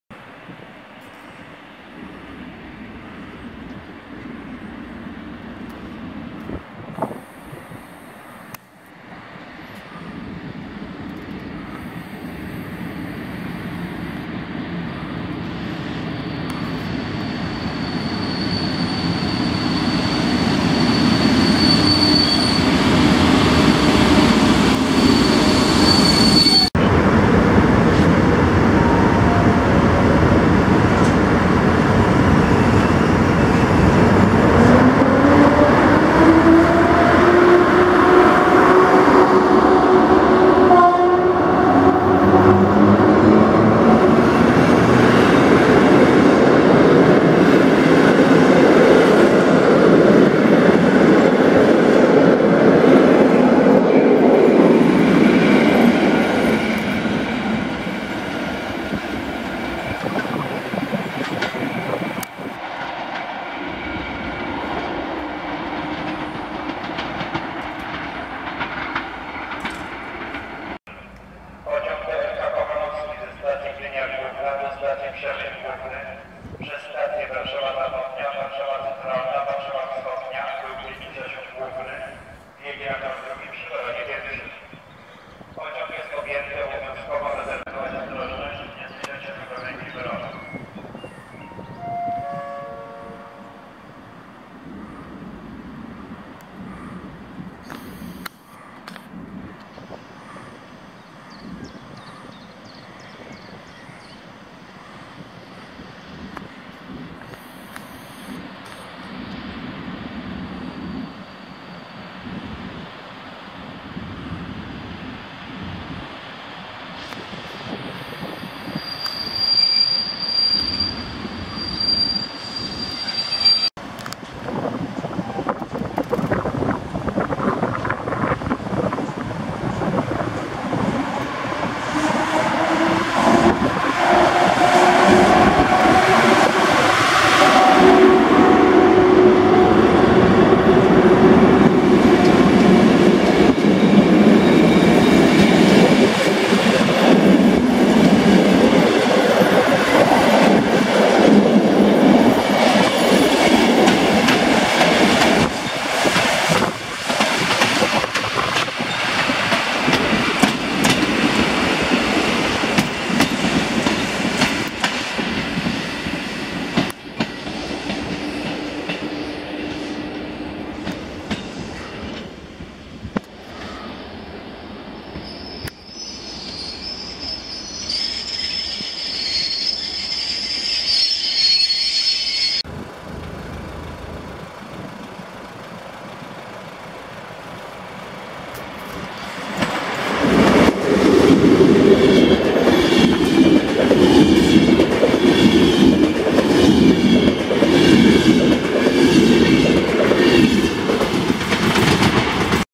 Pociągi na stacji Łowicz Główny 24.04.2017.
EP07-448 z TLK Moniuszko do Bydgoszczy.
EP07-442 z TLK Kochanowski do Przemyśla.
EN57-1252 Regio kończy bieg po czym wróci od razu do Skierniewic.
EP09-040 z EIC Lech do Zielonej Góry.